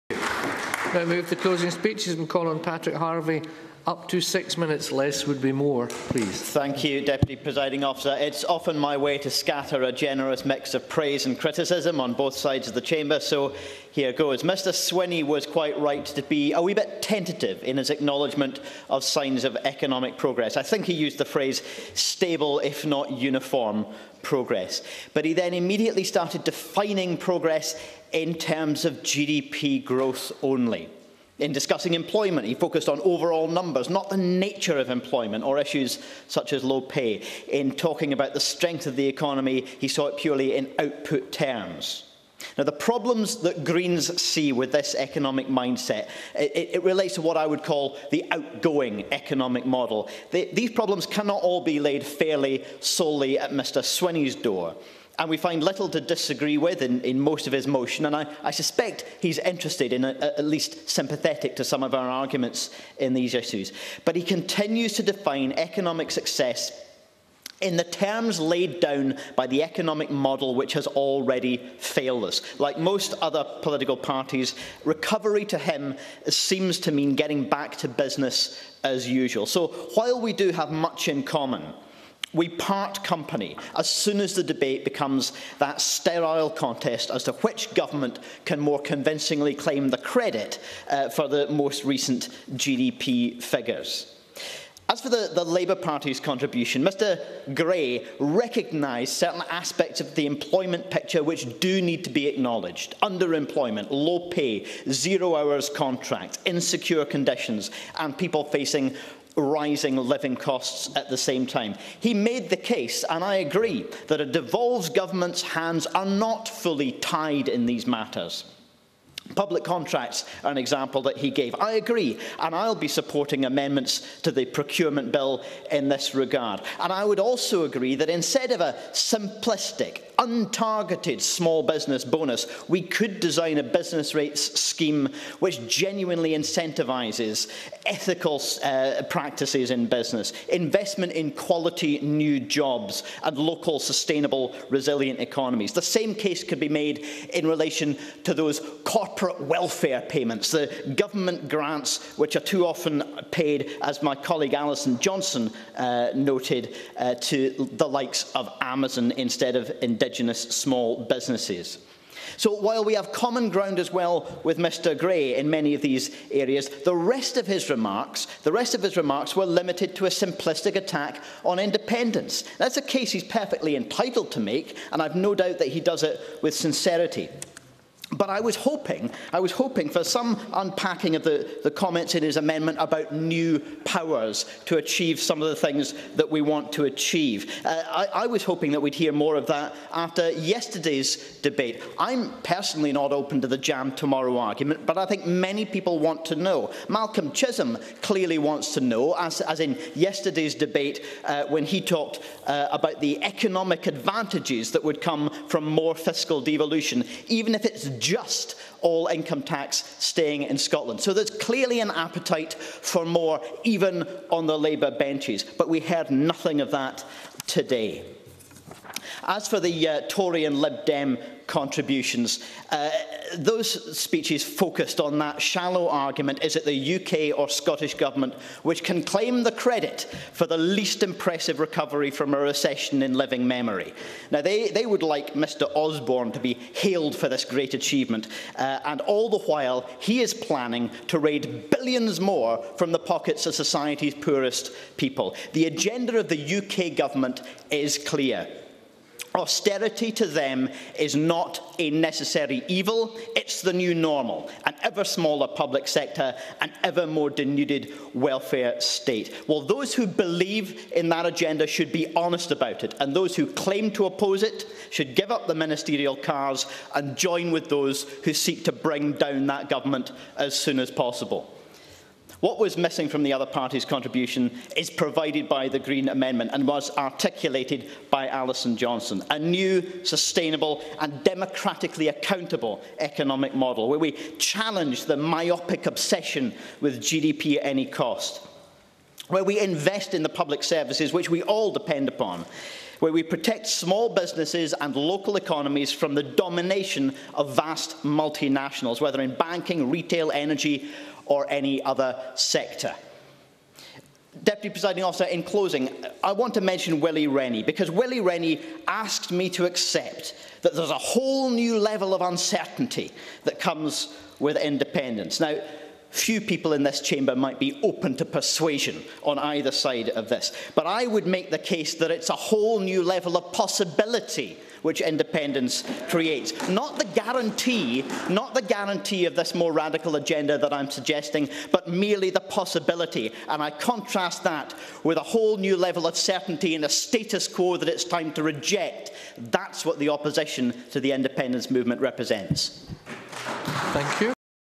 Economy debate 080114
Patrick Harvie MSP speaking in a debate on Scotland's Economy on Wednesday 8th January 2014.